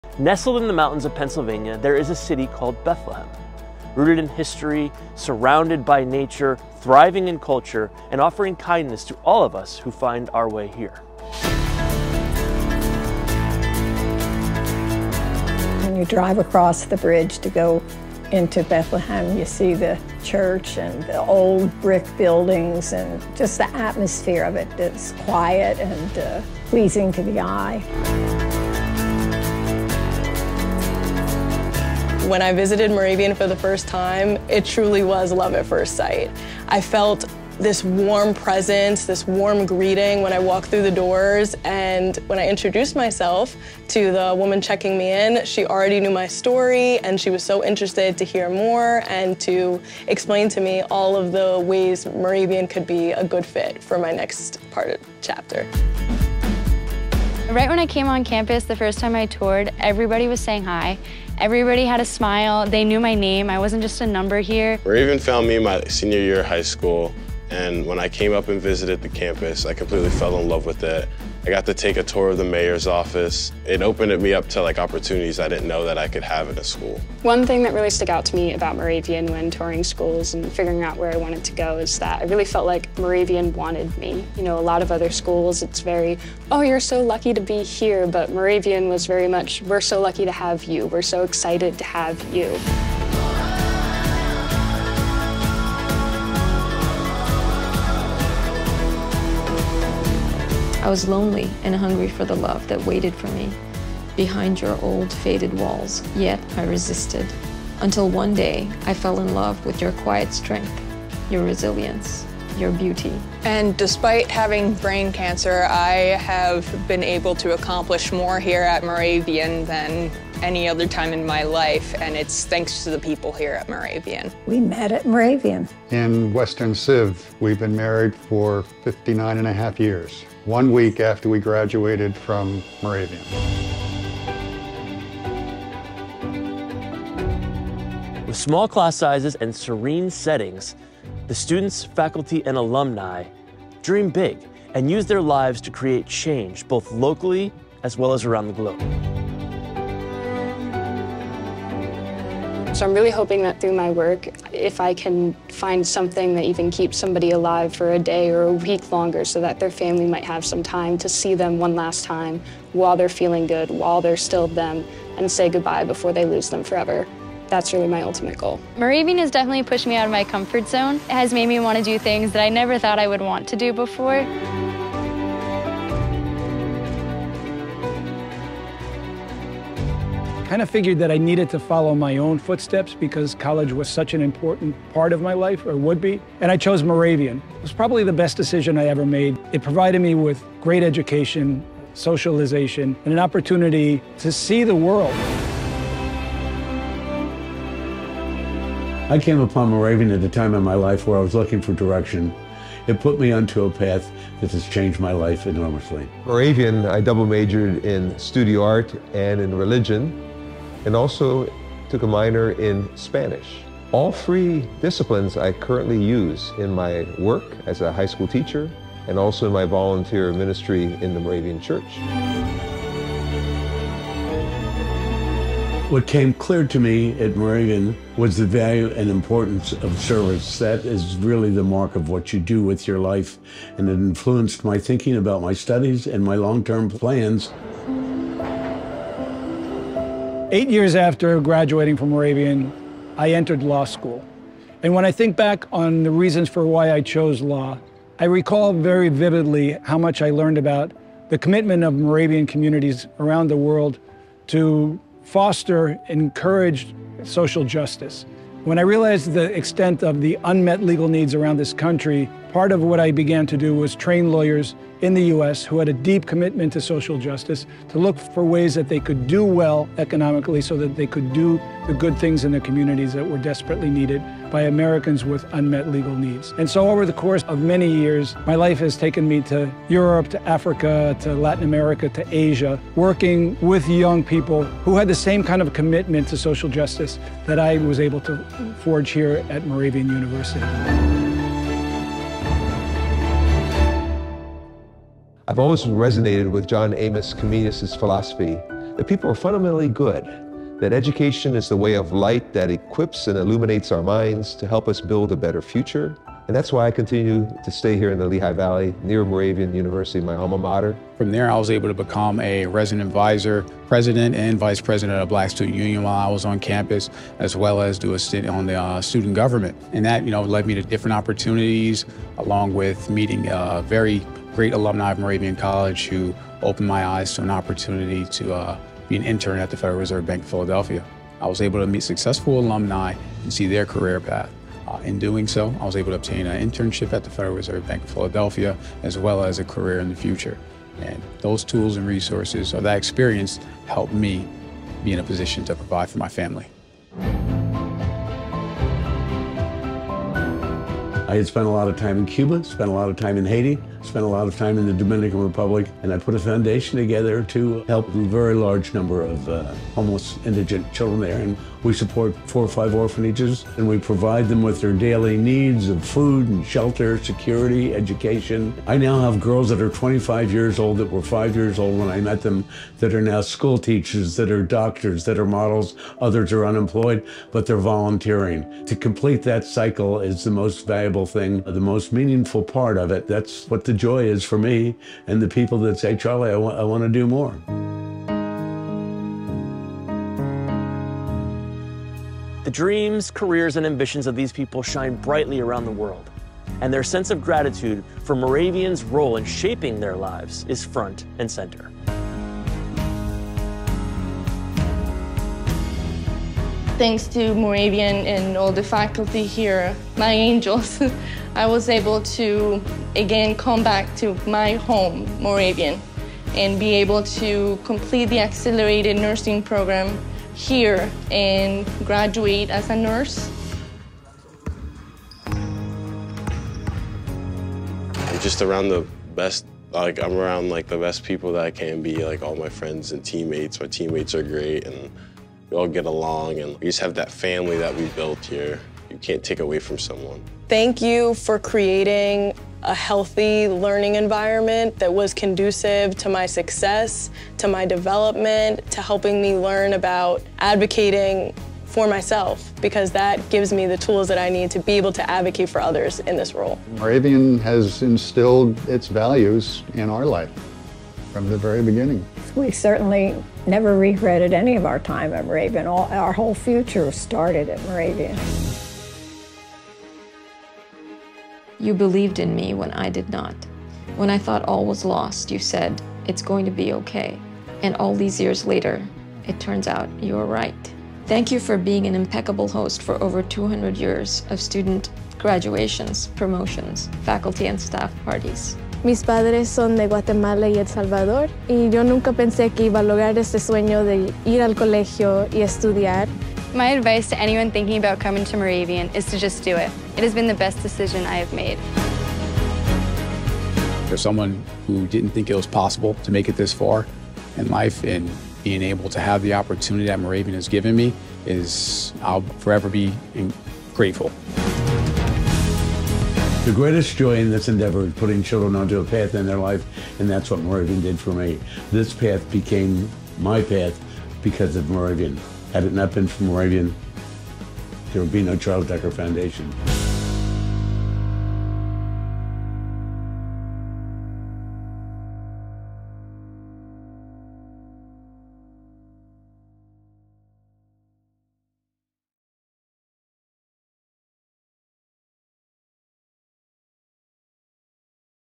Love Letters to Moravia Trailer -Brian intro
"Love Letters to Moravian University" Profiles of several alums about the impact of Moravian o their lives